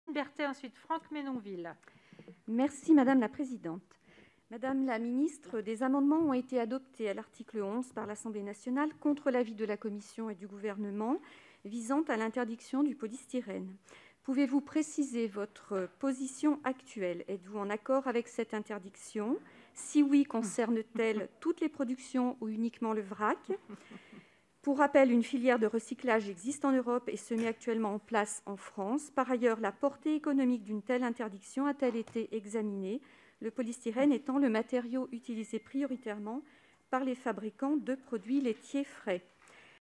Martine Berthet, Sénatrice de la Savoie - Filière Polystyrène : question à Mme la Ministre
La Commission des Affaires Économiques au Sénat, dont Madame la Sénatrice Martine Berthet est membre, a auditionné le 11 mai 2021, Madame la Ministre de la Transition Écologique, Barbara Pompili, dans le cadre du Projet de Loi portant Lutte contre le dérèglement climatique.

A cette occasion, Madame la Sénatrice a interpellé Madame la Ministre sur le sujet de la filière polystyrène.